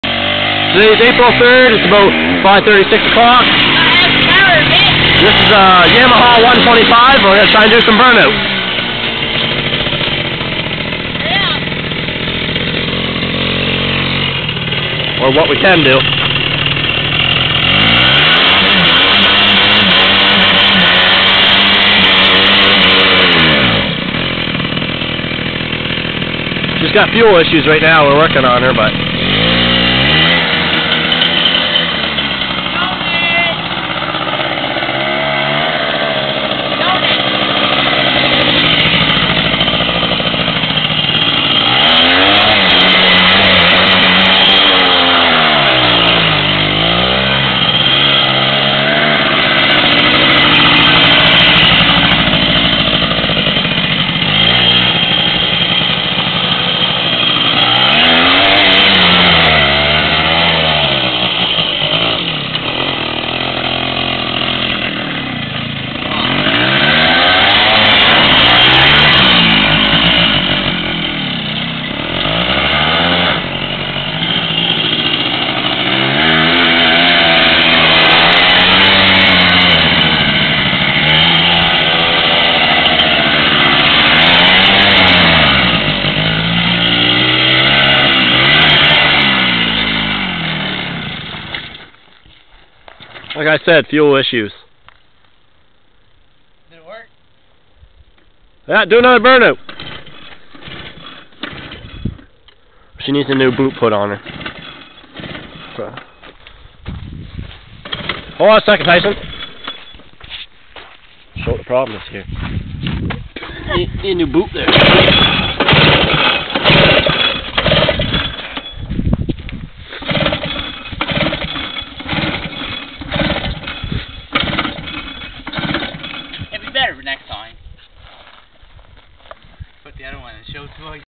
yamaha 125 3 wheeler burnouts and donuts
doing burnouts on dry pavement with a yamaha 125 3wheeler